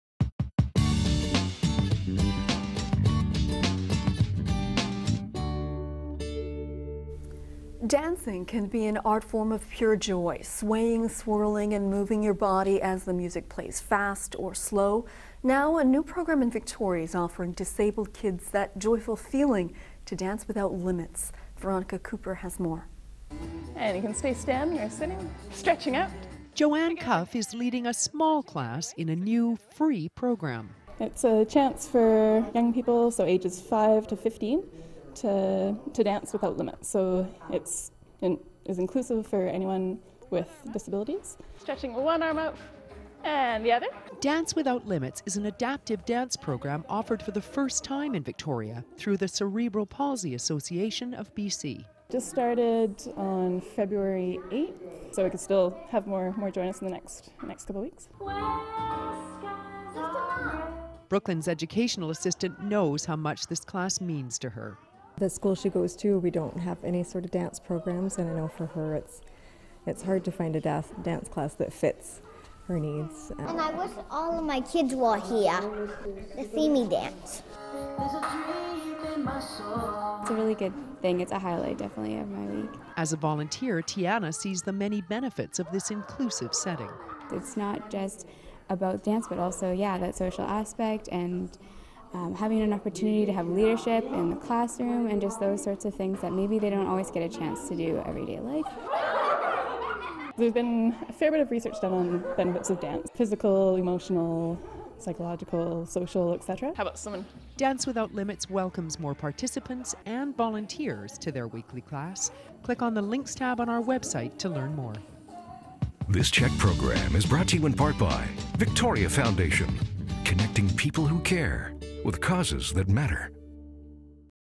Vital People: Dance Without Limits
Dance Without Limits is an adaptive dance program being offered for the first time in Victoria through the Cerebral Palsy Association of BC.  The program allows children with disabilities ages 5 through 15 to enjoy the benefits of dance.